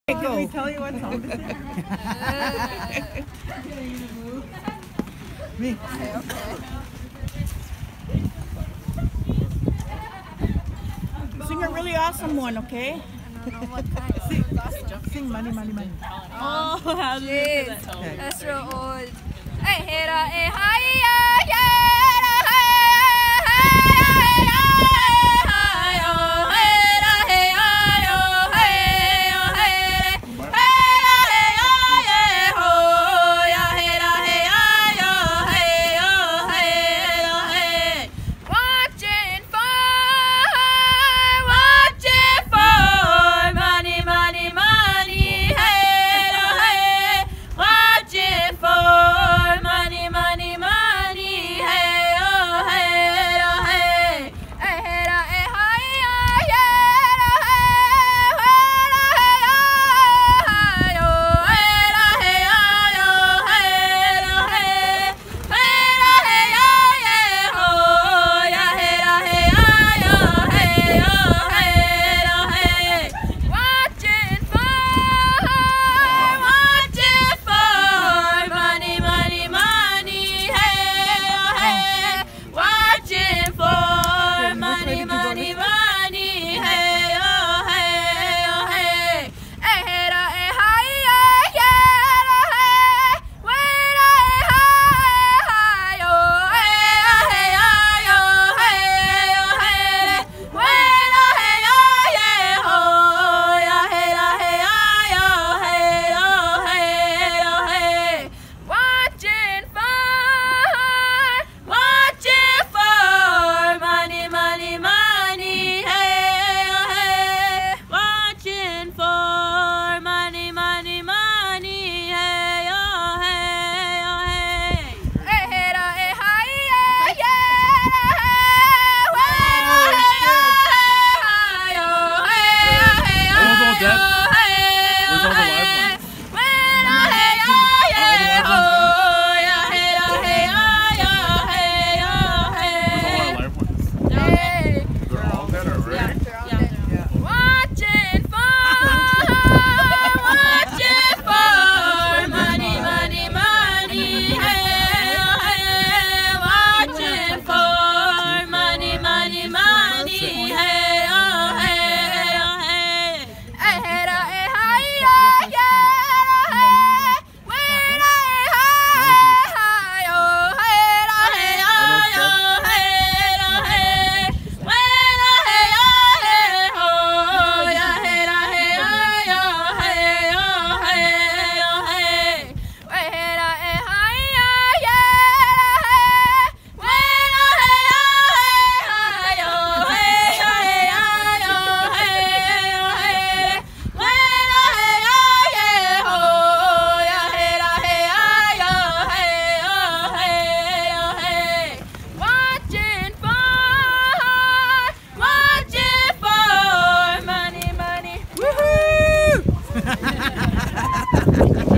Handgame Song by Joslyn and Jeslyn
Recorded in Rosebud,SD. - Aug 26/17 - This girl has an amazing voice and sings with passion!